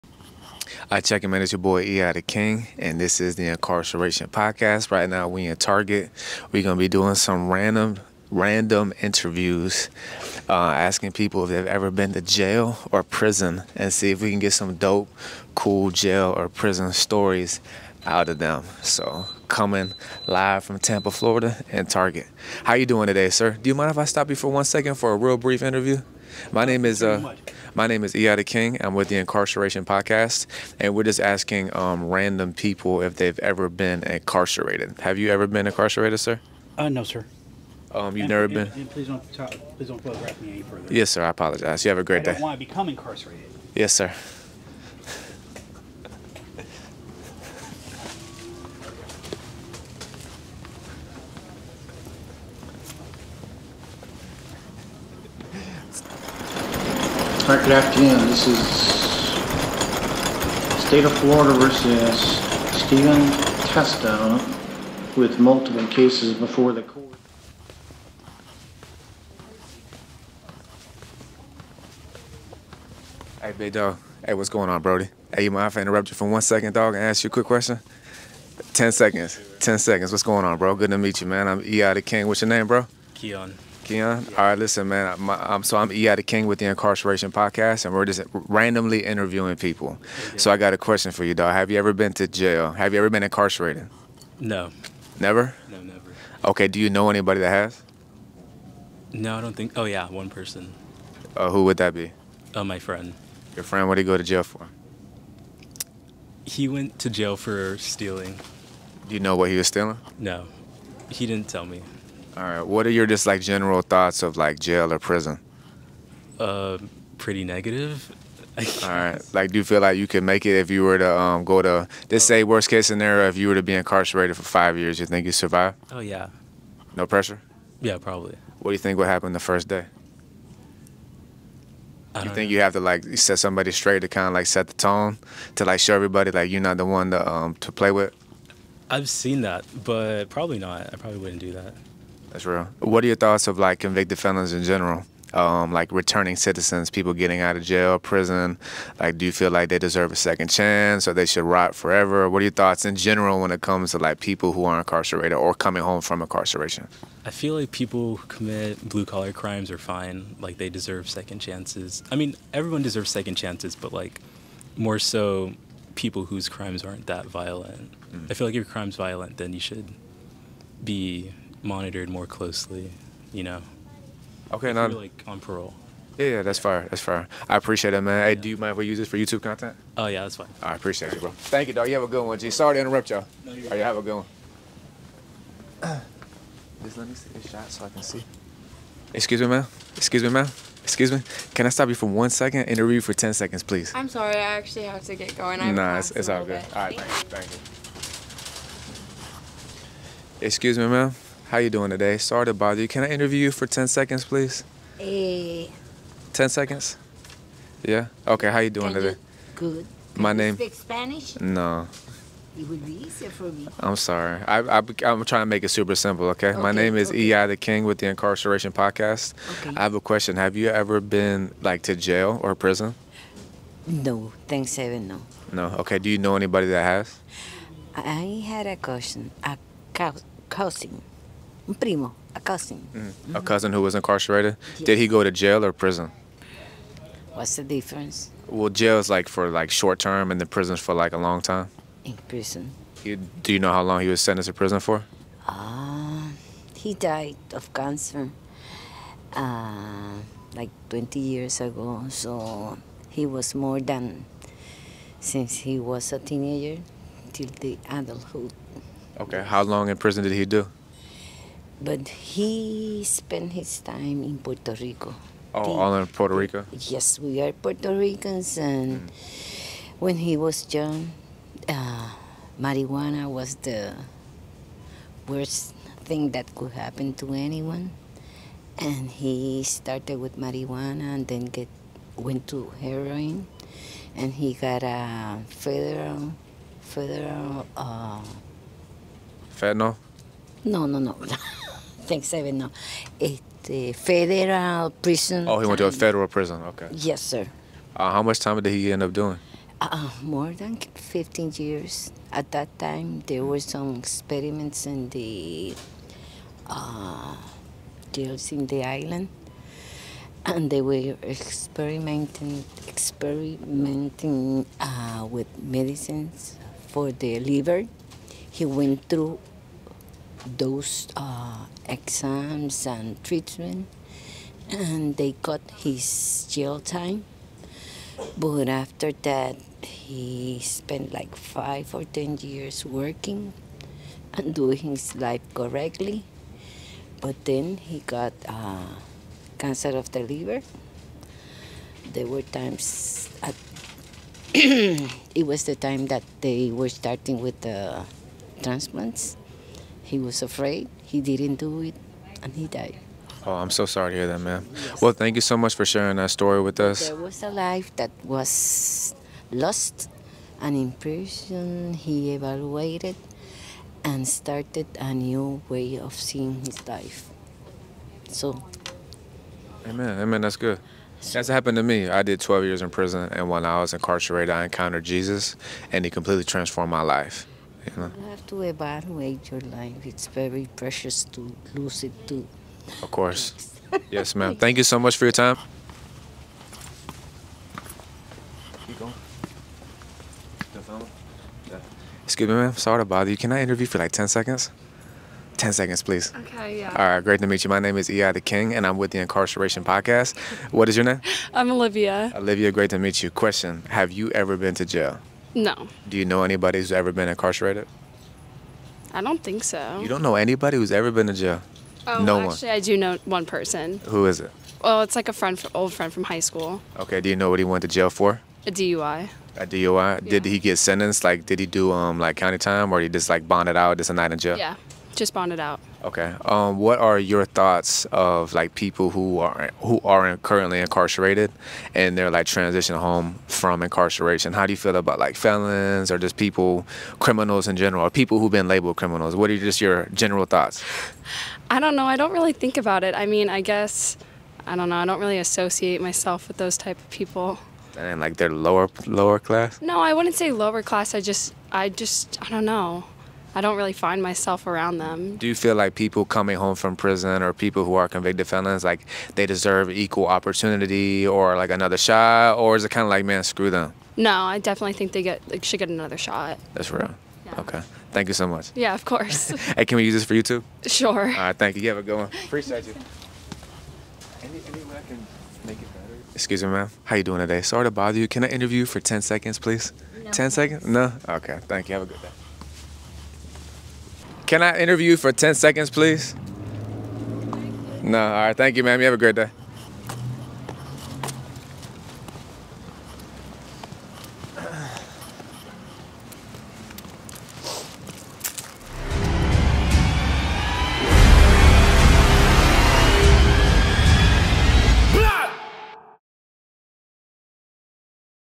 PRISON STORIES INTERVIEW IN TARGET FAIL | Episode 28
E.i. the King tries something different and does random interviews with people in Target, talking about prison, prison stories, etc.

FAIL...

00:00 Old Man Denies Interview
1:20 Awkward Interview
3:09 Girl Denies Interview
3:20 Old Woman Interview
6:31 Woman Interview
8:02 Two More Rejections